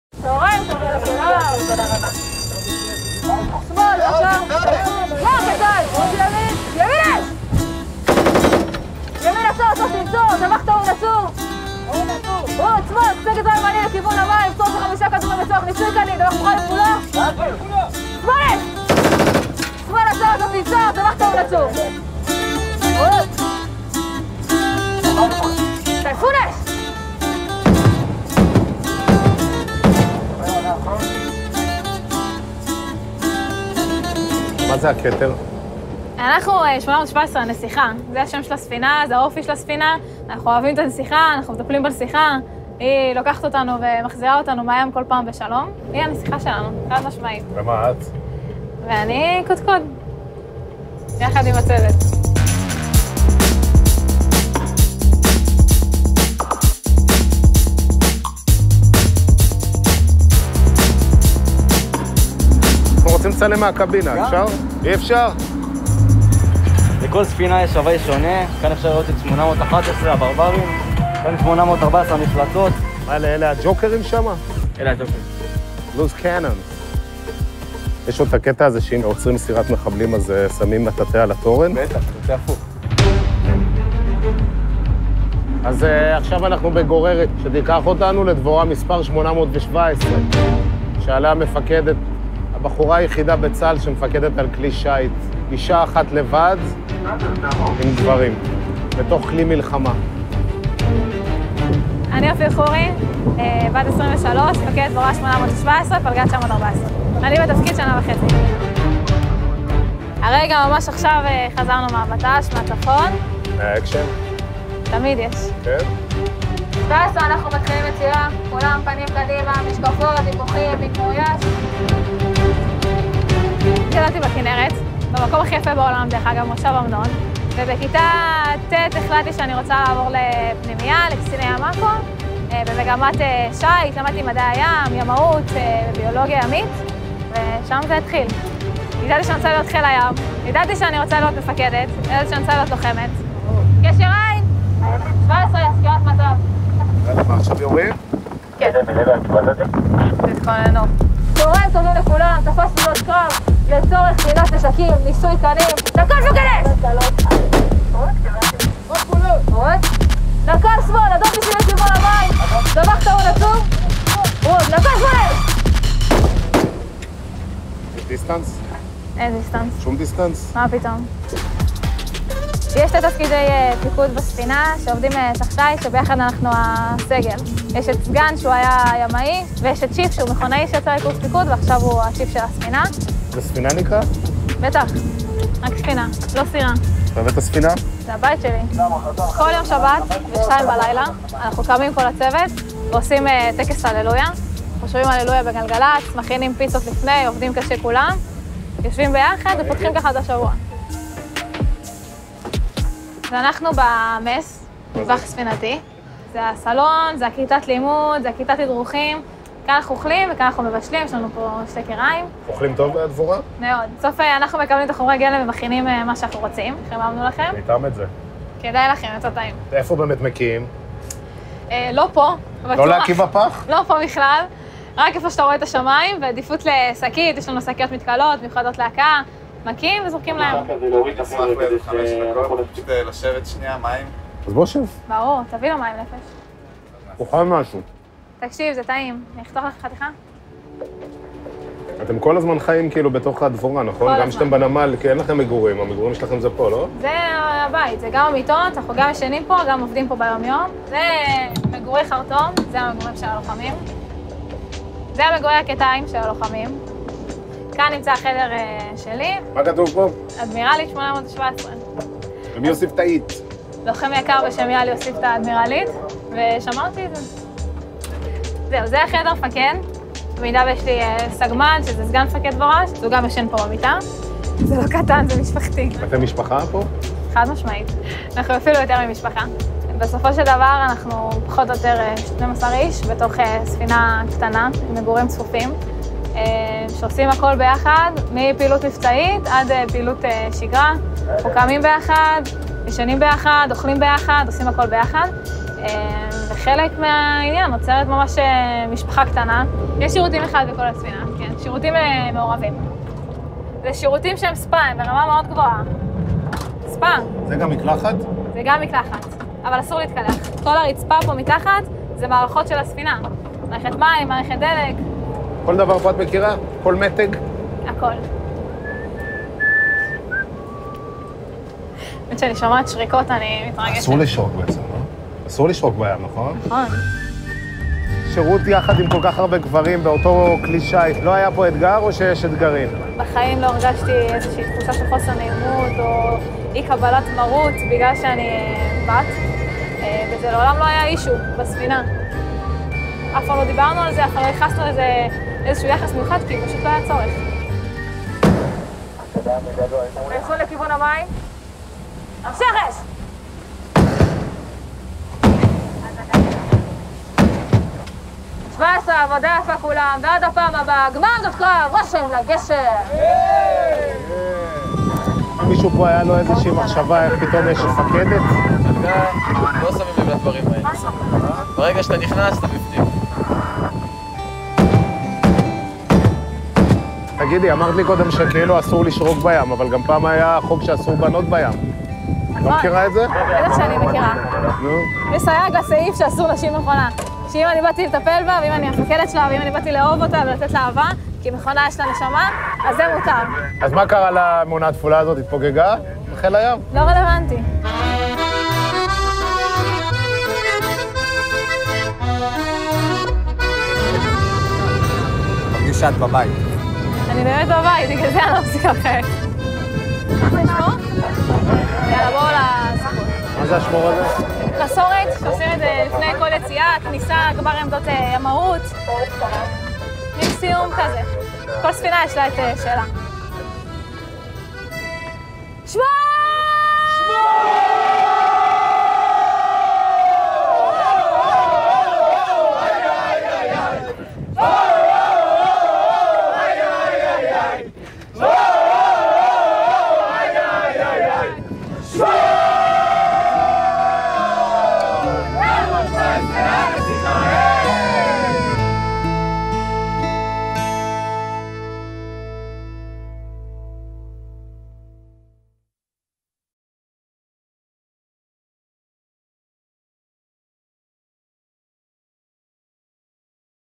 A casa de Ofir: um barco da marinha israelense
A capitã Ofir Khoury, de 23 anos, cresceu com o Mar da Galileia como pano de fundo e sabia desde cedo que queria ser comandante e combatente da marinha. Hoje ela é a única mulher nas Forças de Defesa de Israel a comandar um navio. Como é para ela servir em um navio com homens?